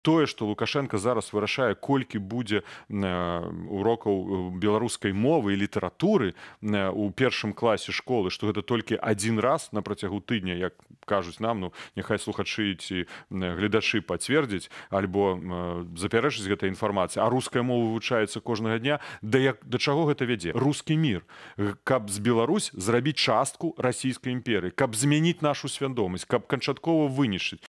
Латушка: Лукашэнка будуе "русский мир" у Беларусі
Кіраўнік НАУ @nau_belarus Павел Латушка @PavelLatushka выказаўся ў эфіры @belsat_news пра жаданне Пуціна і Лукашэнкі пабудаваць "русский мир" у Беларусі і канчаткова знішчыць беларускую свядомасць.